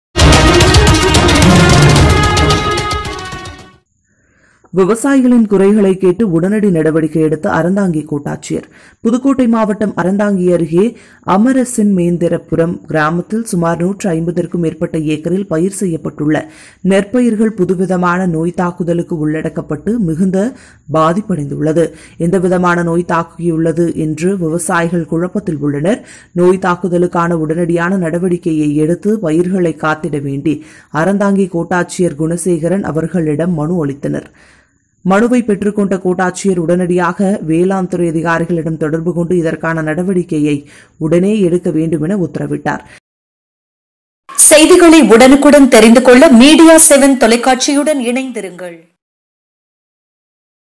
விவசாயிகளின் குறைகளைக் கேட்டு உடனடி நடவடிக்கை எடுத்த அறந்தாங்கி கோட்டாட்சியர்.
புதுக்கோட்டை மாவட்டம் அறந்தாங்கி அருகே அமரசிம்மேந்திரபுரம்
கிராமத்தில் சுமார் 150க்கும் மேற்பட்ட ஏக்கரில் பயிர் செய்யப்பட்டுள்ள நெற்பயிர்கள் புது விதமான நோய் தாக்குதலுக்கு உள்ளாக்கப்பட்டு மிகுந்த பாதிப்படைந்துள்ளது.

எந்தவிதமான நோய் தாக்கியுள்ளது என்று விவசாயிகள் குழப்பத்தில் உள்ளனர் நோய் தாக்குதலுக்கான உடனடியாக நடவடிக்கை எடுத்து பயிர்களை காத்திட வேண்டி அறந்தாங்கி கோட்டாட்சியர் குணசேகரன் அவர்களிடம் மனு அளித்தனர்.

மனுவைப் பெற்றுக் கொண்ட கோட்டாட்சியர் உடனடியாக வேளாண்துறை அதிகாரிகளிடம் தொடர்பு கொண்டு இதற்கான நடவடிக்கையை உடனே எடுக்க வேண்டும் என உத்தரவிட்டார்.

இதுகுறித்து அப்பகுதிவிவசாயி
ஒருவரிடம் கேட்டபொழுது.

 நான் சுமார் 6 ஏக்கர் நிலம் பயிரிட்டுள்ளேன்  பயிர்கள் கதிர் முளைக்கும் நேரத்தில் புதுவிதமான பூச்சிகளால் தாக்கப்பட்டுள்ளது.

இந்நோயின் பெயர் யானை கொம்பன் எனும் நோய் தாக்கப்பட்டு உள்ளது.

இதனால் பயிர்கள் விளைச்சல் ஆகாது நாங்கள் இந்த விவசாயத்தை நம்பித்தான் எங்களுடைய வாழ்க்கை பொருளாதாரம் சார்ந்து உள்ளது.

இந்நோய் தாக்குதல் குறித்து உரம் விற்பனையாளர்களிடம்  அதற்குண்டான மருந்தை கேட்டால் இந்நோய்க்கான மருந்தே கிடையாது என்று கூறுகிறார்கள்.

எனவே இதனை வேளாண்துறை அதிகாரிகள் நேரடியாக பார்வையிட்டு பூச்சி தாக்குதலில் இருந்து பயிர்களை காப்பாற்று எந்தவிதமான மருந்து அடிக்க வேண்டும் என தெரியப்படுத்த வேண்டும்.

 இந்த பாதிப்பில் இருந்து பாதுகாப்பாக பயிர்களை காத்திட வேண்டும் என அறந்தாங்கி கோட்டாட்சியர்  குணசேகரன் அவர்களிடம் மனு அளித்தோம் வேளாண் துறை அதிகாரிகளிடம் பேச்சுவார்த்தை நடத்தி உடனடி நடவடிக்கை எடுக்க வேண்டும் என உறுதி அளித்தபின் நாங்கள் ஆறுதல் அடைந்தோம்.

 மேலும் இந்த இழப்பிற்கான நிவாரண தொகையை அரசு முழுமையாக எங்களுக்கு வழங்கிட வேண்டும் என கோரிக்கை வைத்தனர்.